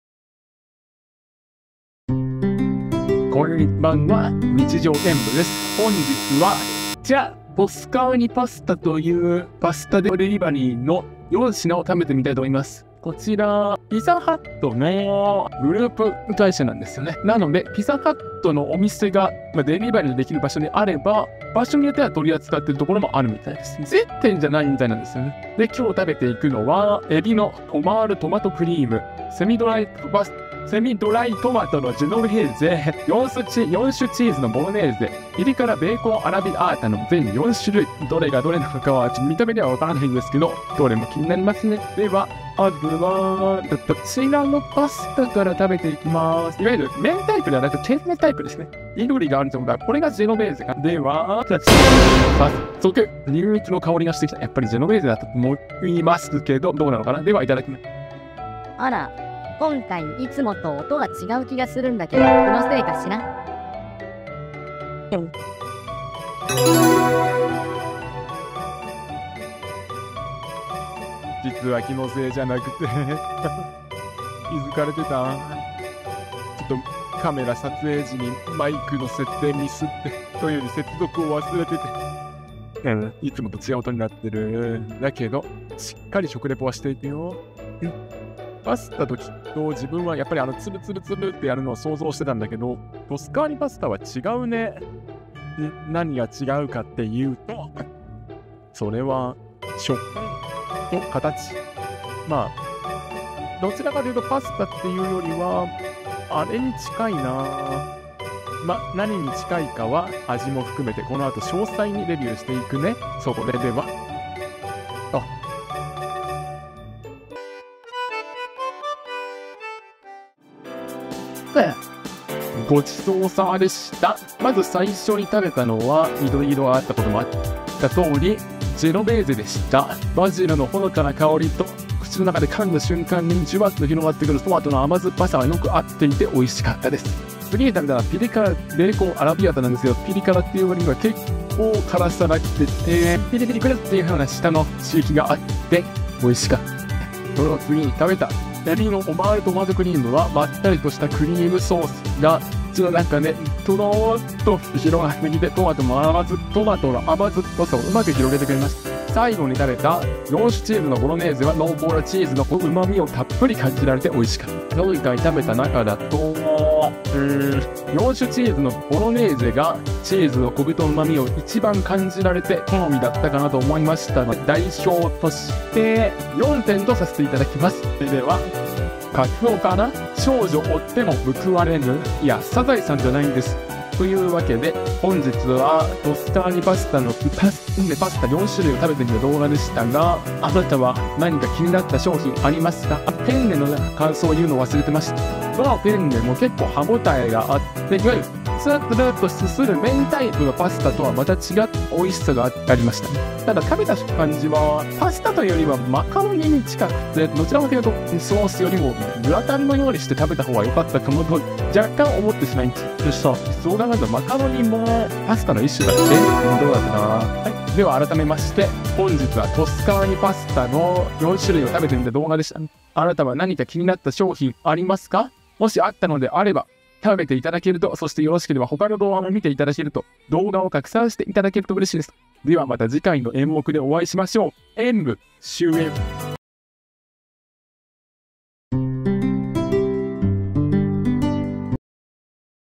ピザ専門店が作ったパスタっておいしいのかな?
【確かにどちらもイタリアだけど~】
ピザ専門店が作ったパスタは、
同じイタリア料理とはいえ、
絶対においしいと言い切れるのかな?

アマゾンほしい物リスト↓